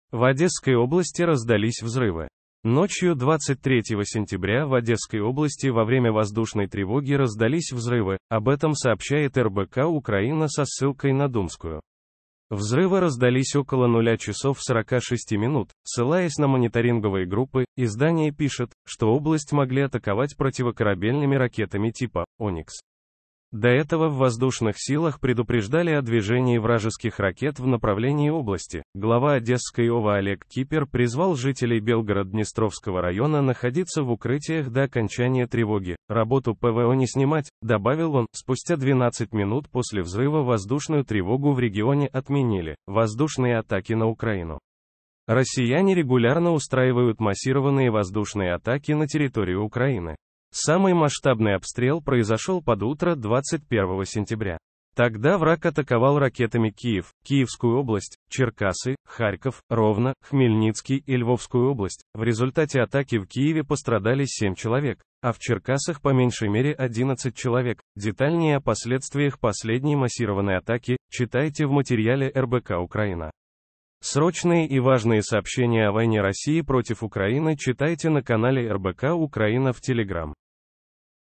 В Одесской области раздались взрывы.
Одесса вибухи ВойнаРоссиипротивУкраины Ночью 23 сентября в Одесской области во время воздушной тревоги раздались взрывы. Об этом сообщает РБК-Украина со ссылкой на Думскую. Взрывы раздались около 00:46, ссылаясь на мониторинговые группы, издание пишет, что область могли атаковать противокорабельными ракетами типа "Оникс". До этого в Воздушных силах предупреждали о движении вражеских ракет в направлении области. Глава Одесской ОВА Олег Кипер призвал жителей Белгород-Днестровского района находиться в укрытиях до окончания тревоги. "Работу